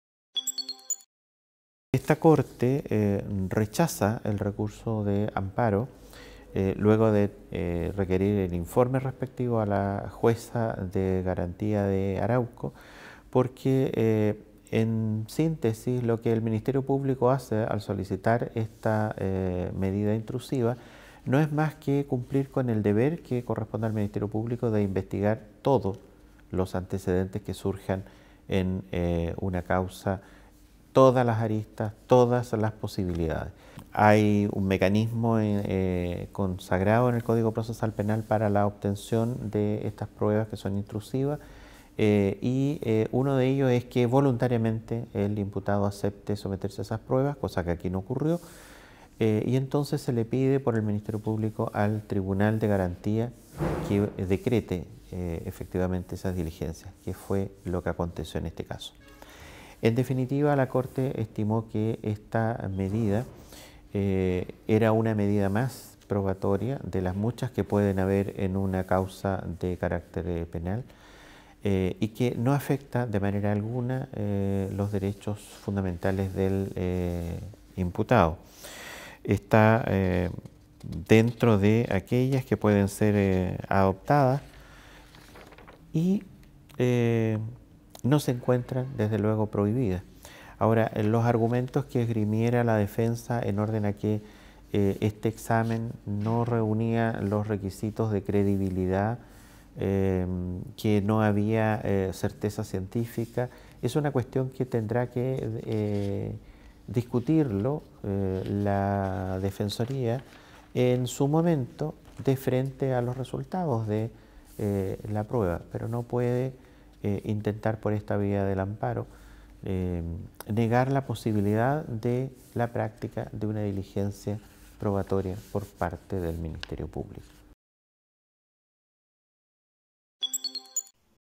Caso Tomás: Corte de Concepción rechaza recurso de amparo y ordena realiza peritaje a imputado
La Corte de Apelaciones de Concepción rechazó hoy –miércoles 1 de diciembre– el recurso de amparo preventivo presentado por la defensa de Jorge Eduardo Escobar Escobar en contra de resolución del Juzgado de Garantía de Arauco que autorizó la realización de exámenes corporales al imputado del homicidio de T.E.B.G.